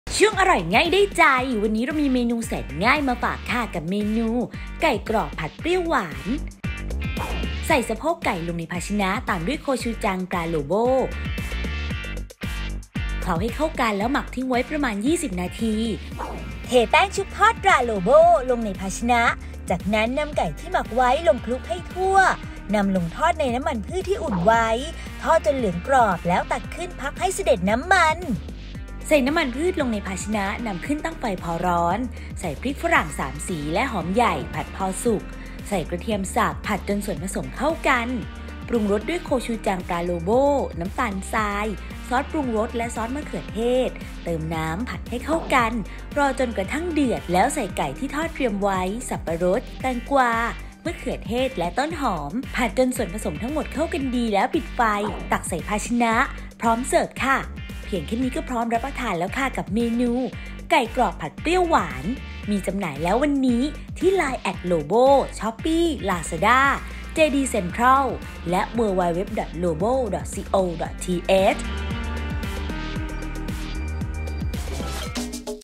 ไก่กรอบผัดเปรี้ยวหวาน สูตรเด็ด ทำง่าย สไตล์ "อ.ยิ่งศักดิ์"
ไก่กรอบผัดเปรี้ยวหวาน
sweet and sour stir fried  Crispy chicken

ผลิตภัณฑ์: โคชูจัง (ซอสพริกเกาหลี) ตราโลโบ

สะโพกไก่หั่นพอดีคำ 250 กรัม
โคชูจัง (ซอสพริกเกาหลี) ตราโลโบ (1)60 กรัม(3 ช้อนโต๊ะ)
แป้งทอดกรอบ ตรา โลโบ 40 กรัม
น้ำมันพืช สำหรับทอดและผัด
พริกฝรั่งสามสีหั่นชิ้นพอดีคำ 80 กรัม
หอมใหญ่หั่นชิ้นพอดีคำ 60 กรัม
กระเทียมสับละเอียด 2  ช้อนชา
โคชูจัง (ซอสพริกเกาหลี) ตราโลโบ (2) 30 กรัม (1 ½ ช้อนโต๊ะ)
ซอสมะเขือเทศ 1  ช้อนโต๊ะ
สับปะรดหั่นชิ้นพอดีคำ 80 กรัม
แตงกวาหั่นเฉียง 30 กรัม
มะเขือเทศหั่นชิ้นพอดีคำ 45 กรัม
ต้นหอมหั่นท่อน 10 กรัม

1. ใส่สะโพกไก่ลงในภาชนะ ตามด้วยโคชูจัง ตราโลโบ (1) เคล้าให้เข้ากัน หมักทิ้งไว้ประมาณ 20 นาที
2. นำไก่ที่หมักไว้คลุกกับแป้งทอดกรอบ ตราโลโบ จากนั้นนำลงทอดในน้ำมันพืชที่อุ่นไว้ ทอดจนเหลืองกรอบ ตักขึ้น พักให้สะเด็ดน้ำมัน เตรียมไว้
3. ใส่น้ำมันพืชลงในภาชนะ นำขึ้นตั้งไฟพอร้อน ใส่พริกฝรั่งสามสีและหอมใหญ่ ผัดพอสุก ใส่กระเทียมสับ ผัดจนส่วนผสมเข้ากัน
4. ปรุงรสด้วยโคชูจัง ตราโลโบ (2) น้ำตาลทราย ซอสปรุงรสและซอสมะเขือเทศ เติมน้ำ ผัดให้เข้ากัน รอจนกระทั่งเดือด ใส่ไก่ที่ทอดเตรียมไว้ สับปะรด แตงกวา มะเขือเทศและต้นหอม ผัดจนส่วนผสมทั้งหมดเข้ากันดี ปิดไฟ ตักใส่ภาชนะ พร้อมเสิร์ฟ

▲Line @yingsakfood